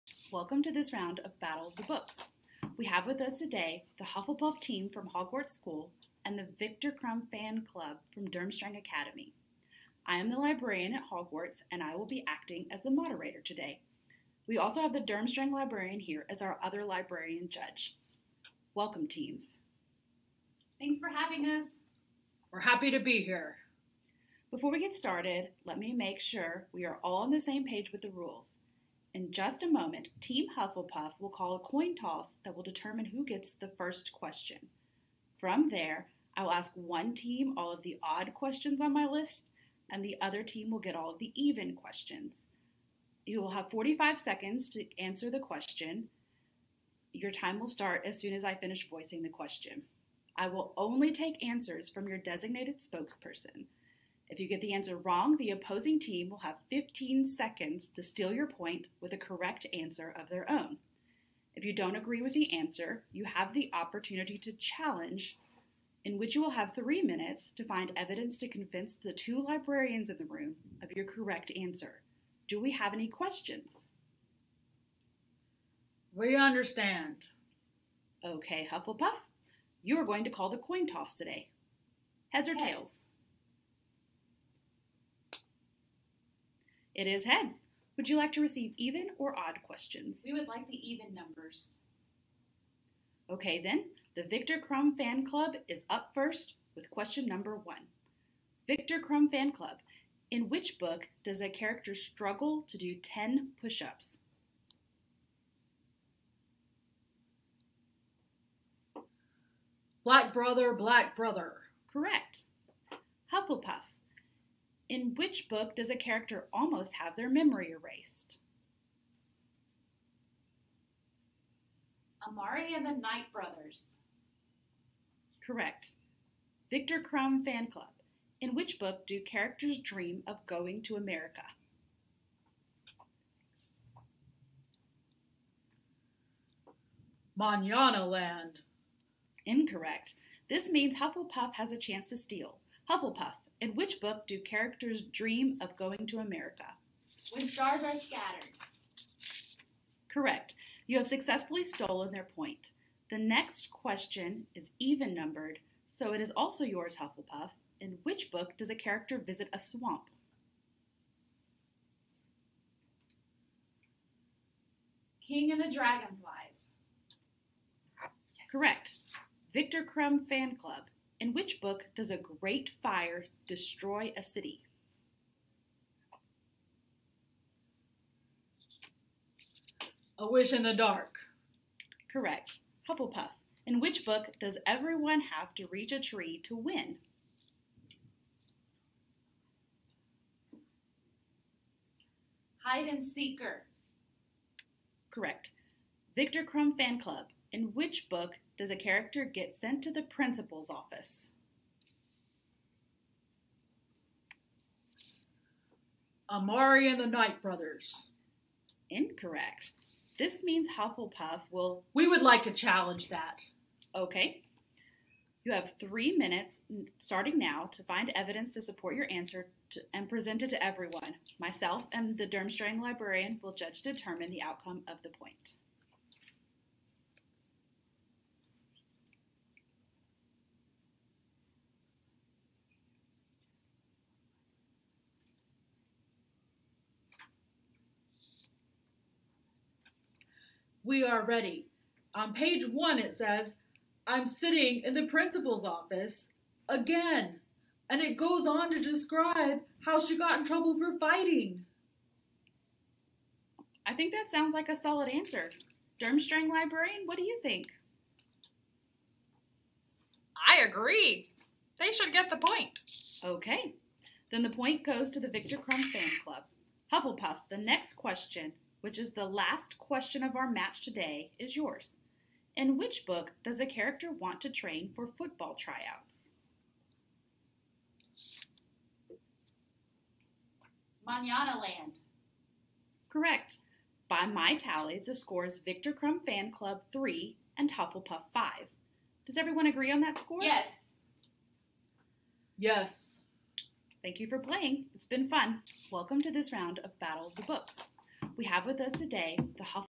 BoB Demo
In this demo, I cloned myself to show you what it looks like! Is the acting great? No. Is the video editing perfect? Also no. But I think it will at least give you an idea of what to expect.